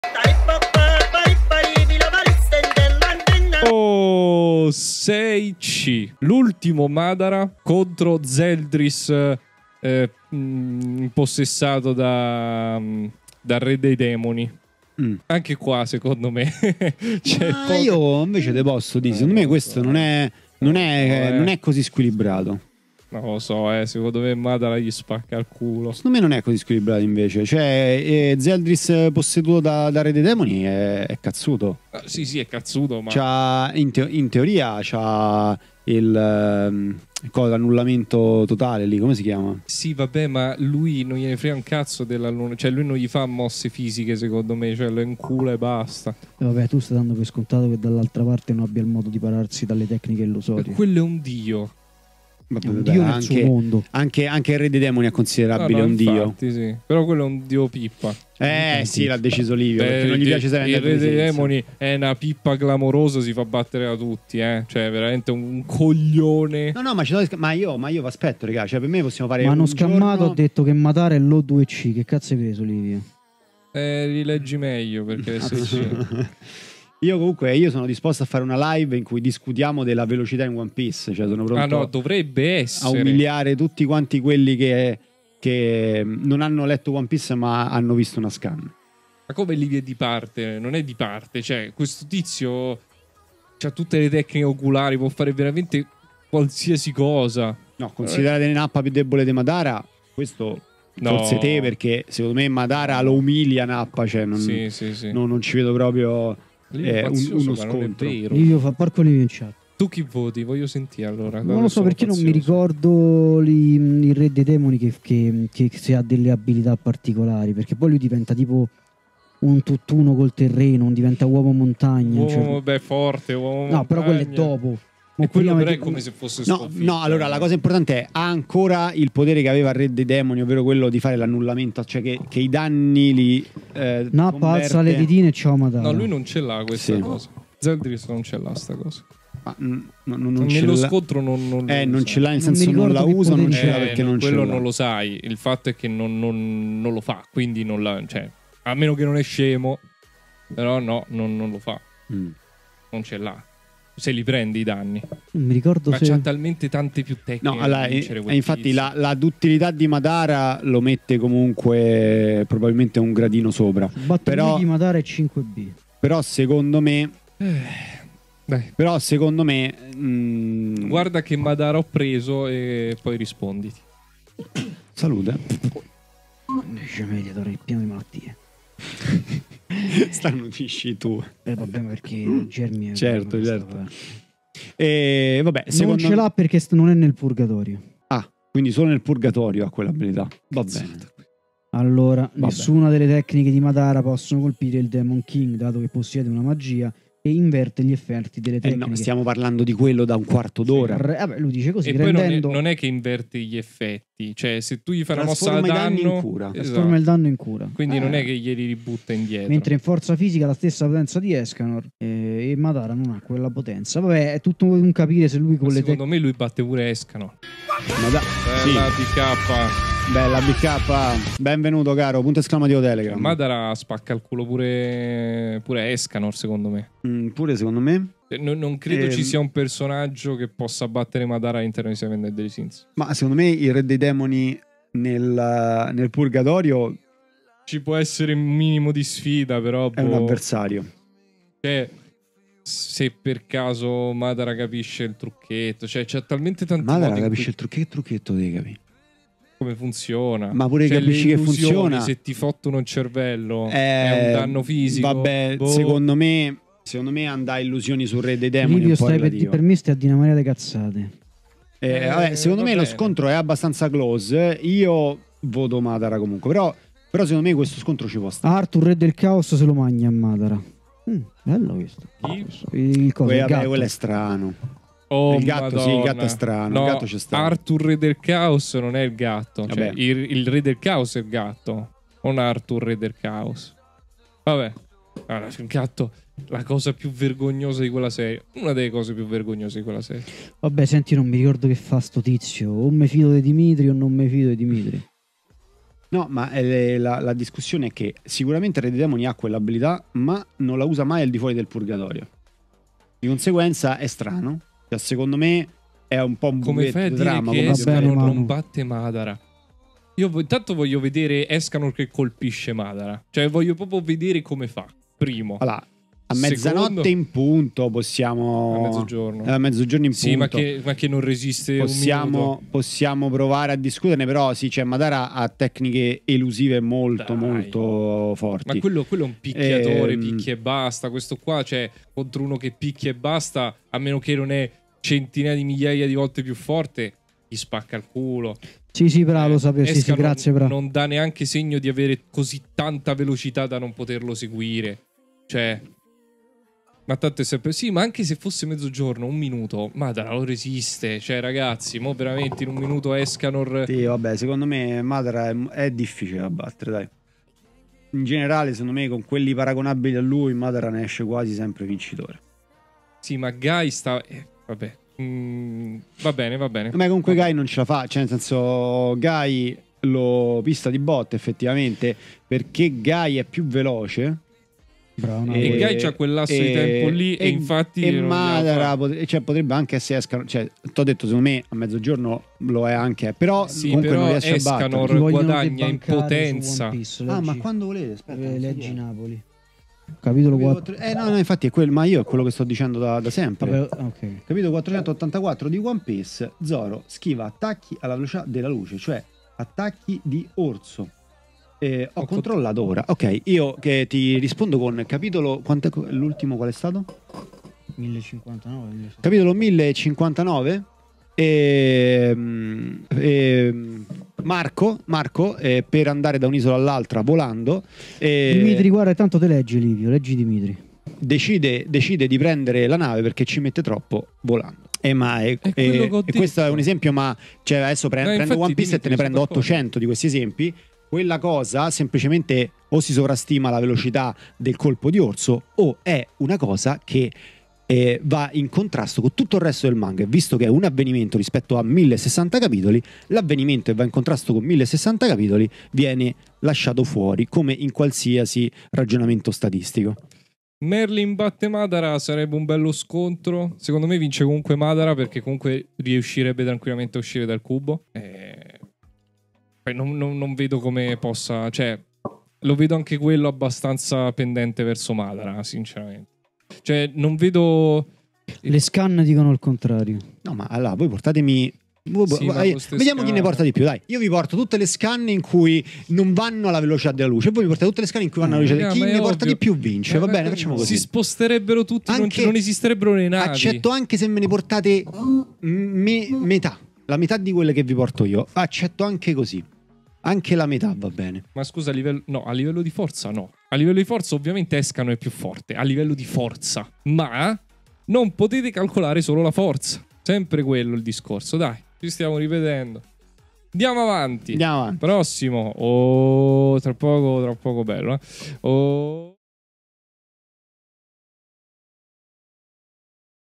LIVELLI DI COMBATTIMENTO: MADARE SAGE MODE vs GOD ZELDRIS - Versus con la SCALA dei POWER SCALER!
Versus fatto sulla scala di Battle Wiki, il punto di riferimento dei Power Scaler! Hanno senso? Chi calcola la potenza, le tecniche, la distruzione e l'inventiva! Chi vincerà alla fine? Vediamo i livelli di combattimento.
Fatemi sapere nei commenti chi secondo voi vincerebbe in uno scontro di potenza.

Se il video ti è piaciuto ti invito a mettere un bel Like!